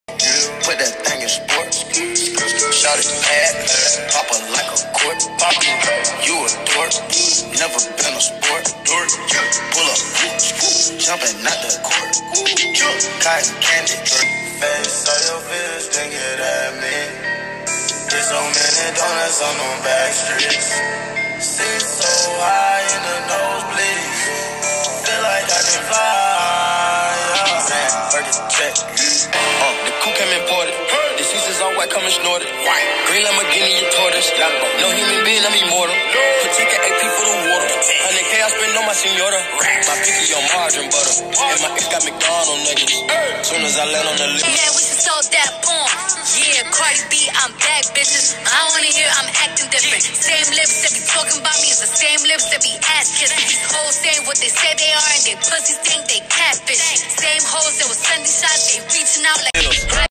CARDI B MUESTRA ANILLO DE COMPROMISO
Este video fue compartido por Cardi B en su cuenta de Instagram, con el siguiente mensaje: "Solo quiero irme a dormir y despertarme y asegurarme de que esto no sea un sueño. Gracias, Dios, por todas estas bendiciones. Gracias, cariño. @offsetyrn, Dios realmente te envió a mí".